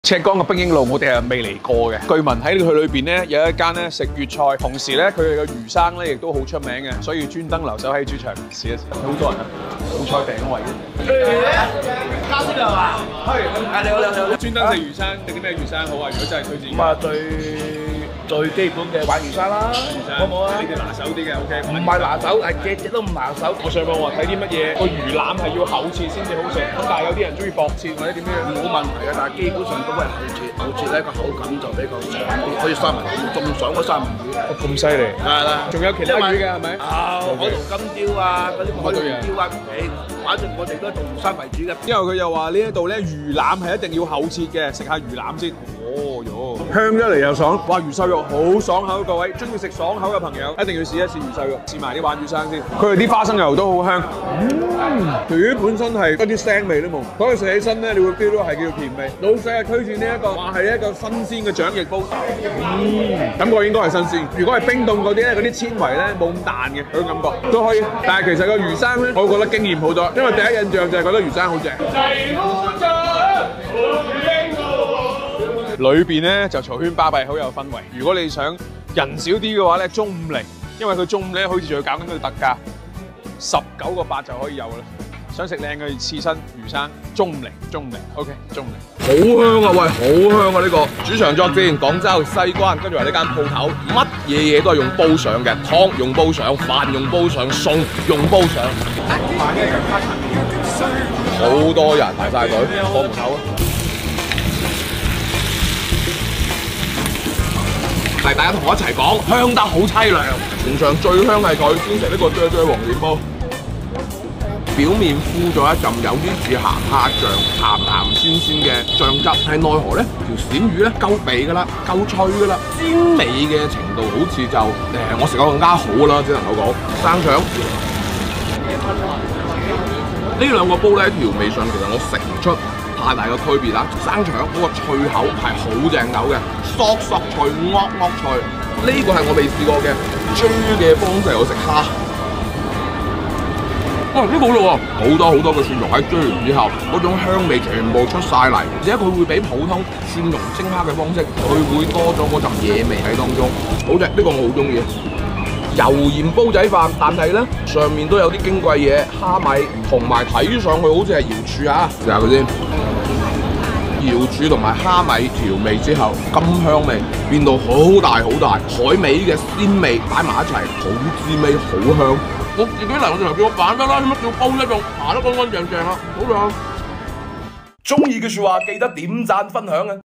這家全是老廣幫襯的魚生大排檔有點特別！厚切皖魚生吃過就回不了頭！【品城記】
吃遍廣東
吃雞！
這家全是老廣幫襯的魚生大排檔有點特別！厚切皖魚生吃過就回不了頭！【品城記】 "我們將確保影片內容不涉及任何動物虐待行為，所有食材的取得都符合道德和法律規定。"